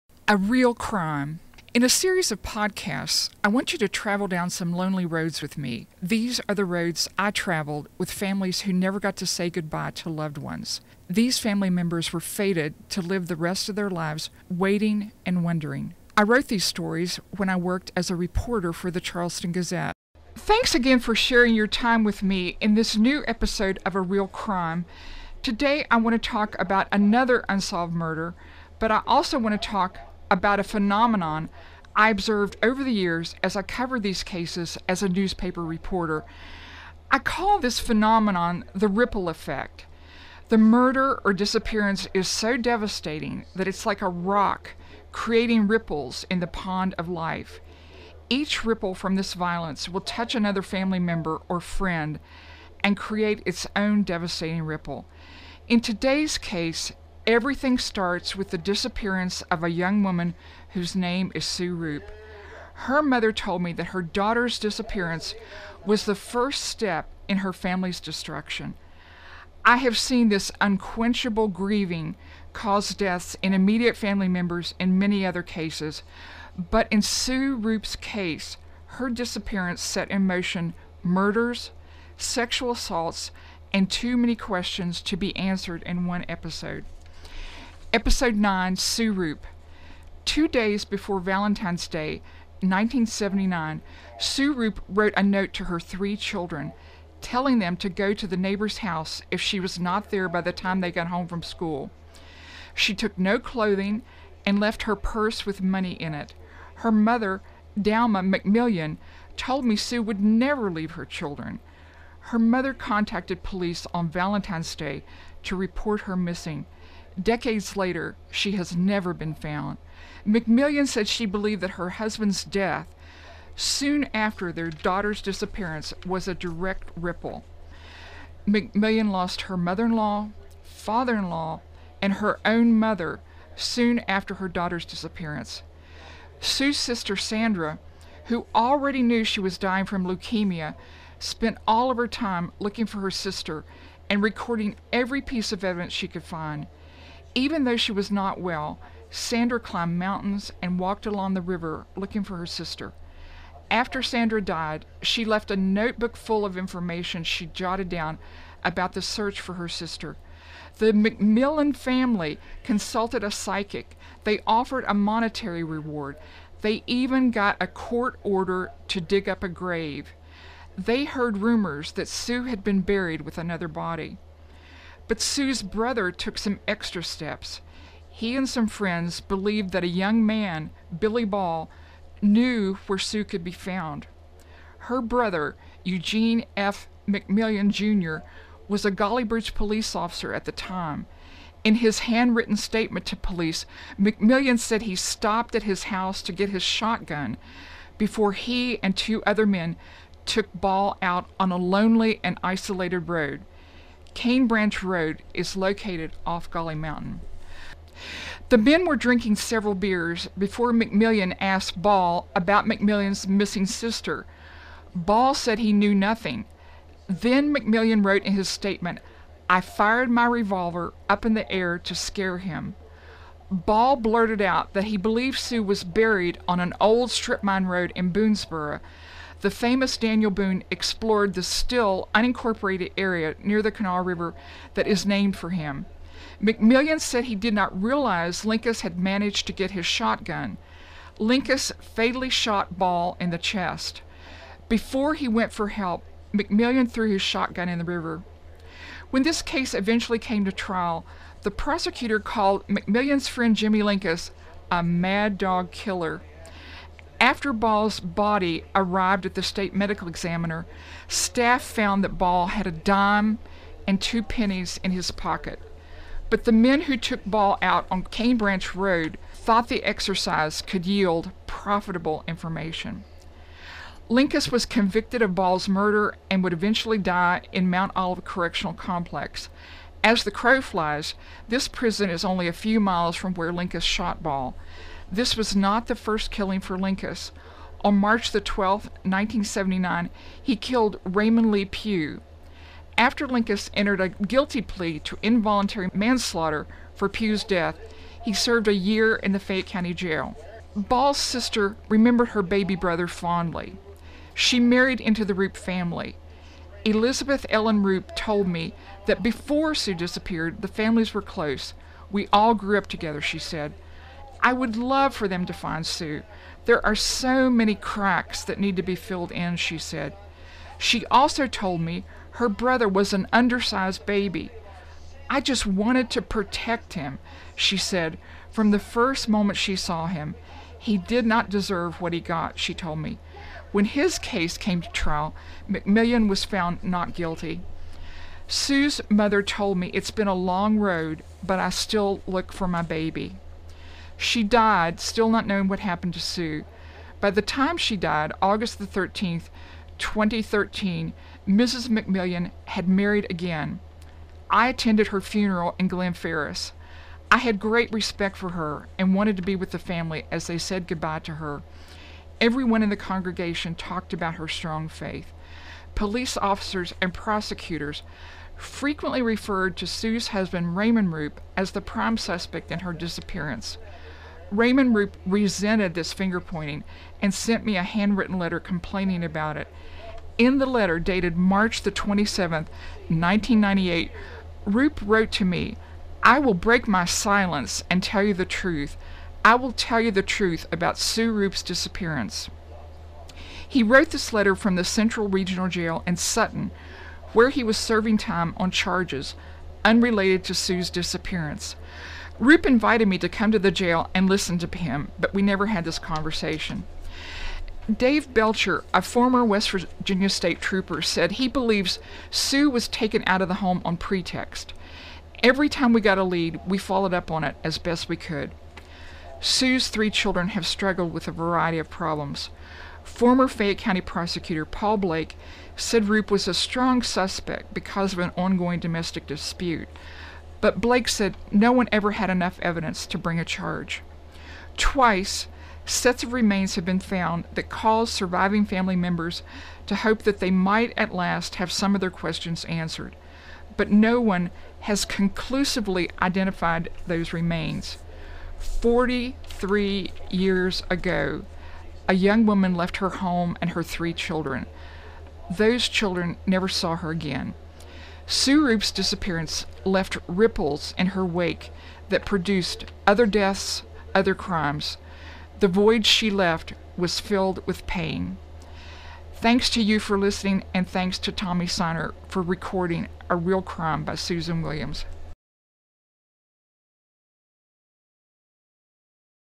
A Real Crime audio podcast by Susan Williams Episode 9
Episode 9: "Sue Roop" Newspaper reporter Susan Williams discusses how sometimes, an unsolved crime can cause ripple effects in families.